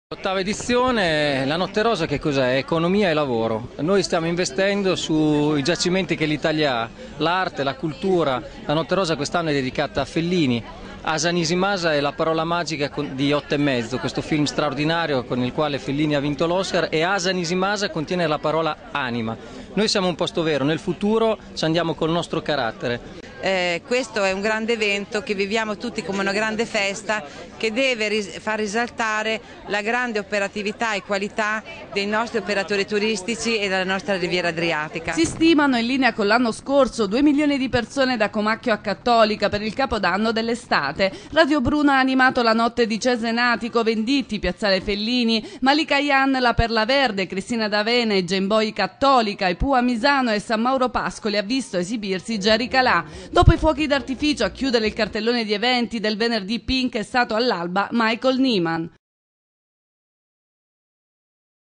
ANDREA GNASSI: UNA NOTTE ROSA ALL'INSEGNA DELLA DOLCE VITA
Anche in questa ottava edizione, la notte rosa è all'insegna dell'arte, della cultura e tutto ciò che la riviera può offrire. In particolare, quest'anno, la notte rosa è dedicata a Fellini, e la sua parola magica è AsaNIsiMAsa, riferita al film 8 e mezzo, con cui Fellini ha vinto l'Oscar...